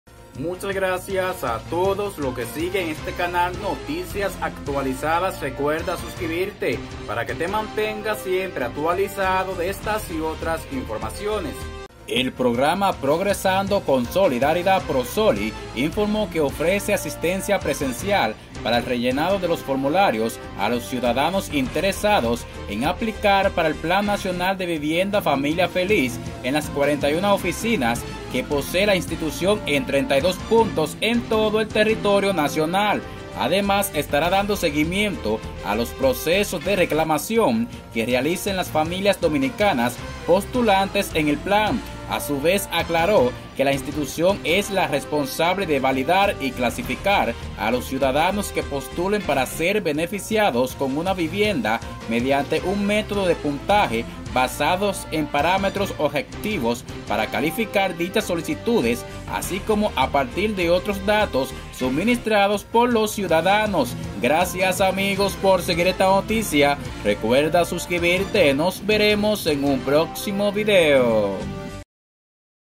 Prosoli brinda asistencia presencial a interesados en Plan Vivienda Familia Feliz
El Programa Progresando con Solidaridad (Prosoli) informó que ofrece asistencia presencial para el rellenado de los formularios a los ciudadanos interesados en aplicar para el Plan Nacional de Vivienda Familia Feliz (PNVFF) en las 41 oficinas que posee la institución en 32 puntos en todo el territorio nacional.

Además estará dando seguimiento a los procesos de reclamación que realicen las familias dominicanas postulantes en el plan.

A su vez, aclaró que la institución es la responsable de  validar y clasificar a los ciudadanos que postulen para ser beneficiados con una vivienda, mediante un método de  puntaje basado en parámetros objetivos para calificar dichas  solicitudes así como a partir de otros datos suministrados por los ciudadanos.

Dentro de las variables y condiciones personales y familiares que se están tomando en cuenta se encuentran: hogares liderados por mujeres, mujeres que hayan sido víctimas de violencia doméstica, personas con discapacidad, madres solteras, edad de las personas postulantes, cantidad de hijos y familias que se encuentren en situación de pobreza extrema y moderada.

En ese orden, en pos de  garantizar que el programa priorice las familias que presenten mayor vulnerabilidad, se establecieron tres grupos de enfoques: familias monoparentales, familias extendidas y familias nucleares.

Adicionalmente, Prosoli estará educando a los  ciudadanos en economía doméstica para que puedan prepararse para acceder a las facilidades de financiamiento y al mismo tiempo   que puedan cumplir con sus corresponsabilidades y los compromisos económicos que asuman.

La institución indicó que, este sistema de evaluación asegura impactar de manera directa a los sectores más vulnerables del país, permitiendo hacer transparente el proceso de selección de los potenciales beneficiarios.

También, se estará realizando la verificación de corresponsabilidades, para asegurar que los participantes del PNVFF cumplan con sus deberes en educación y salud, así como el pago de las cuotas mensuales y habitar el inmueble por un período de 10 años (no puede ser alquilado, vendido y/o donado).

PNVFF ofrecerá acompañamiento para que todos los ciudadanos puedan acceder a facilidades de financiamiento mediante el otorgamiento de subsidios e incentivos al ahorro permitiendo incluso a los más pobres acceder al  mercado de vivienda.

Se recuerda que el pasado 16 de abril el Presidente de la República dejó iniciado de manera simultánea los trabajos de 09 proyectos habitacionales para un total de 3,048 apartamentos distribuidos en territorios en diferentes puntos de la geografía nacional en esta primera entrega.